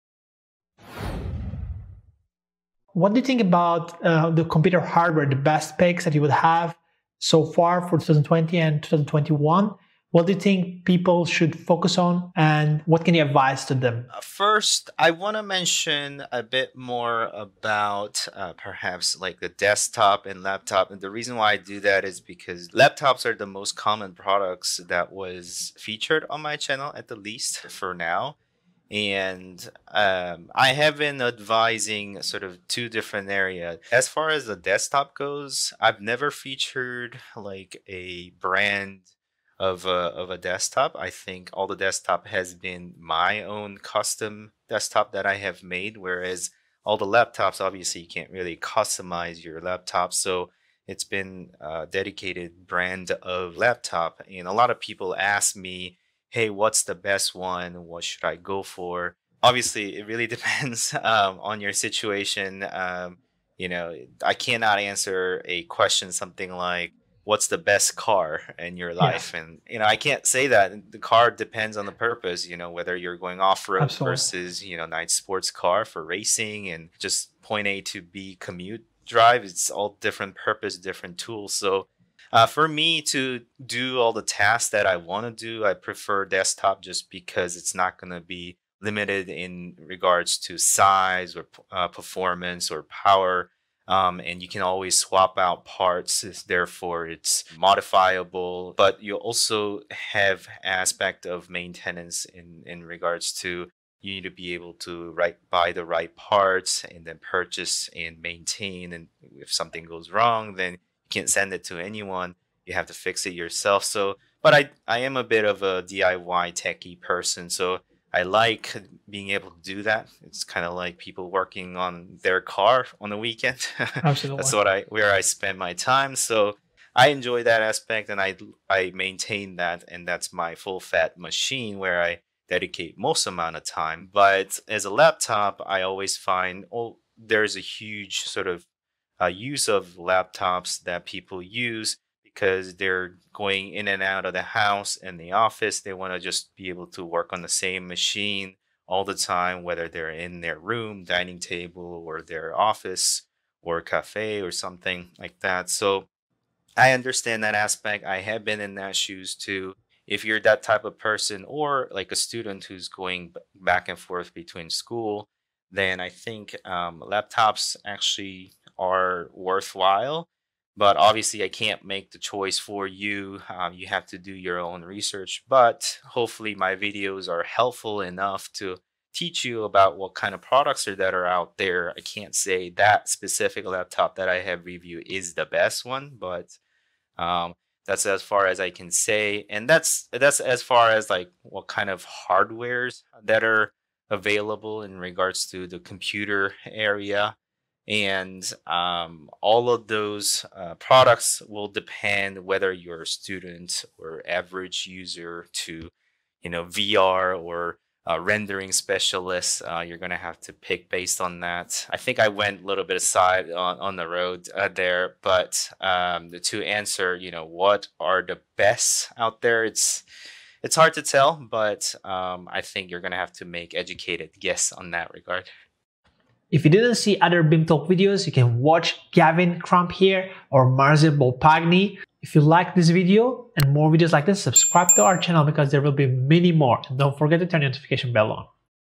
Why you should use a Desktop NOT a Laptop for BIM
It is better to use a Desktop NOT a Laptop for BIM. Do you know why? Joseph Kim is answering that question in this video.

Watch until the end to understand why he prefers to work on a Desktop computer instead of on a Laptop, although on his videos he is mainly reviewing Laptops.

This is a more Technical BIM talk from which you will learn what could be the best solution for you depending on your purposes.

We've prepared special selection series of the most interesting moments from our BIM Talks with different BIM professionals.